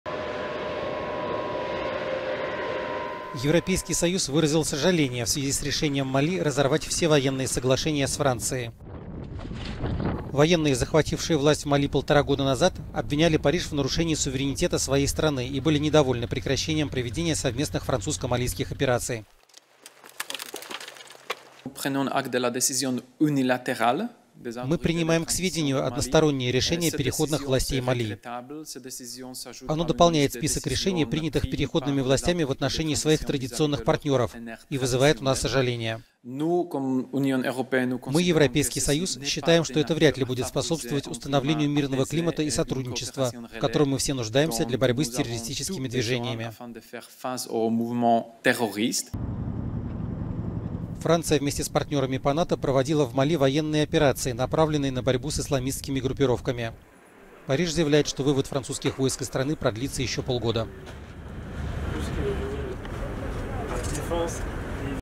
ЕС сожалеет о решении Мали
Евросоюз выразил сожаление в связи с решением Мали разорвать все военные соглашения с Францией.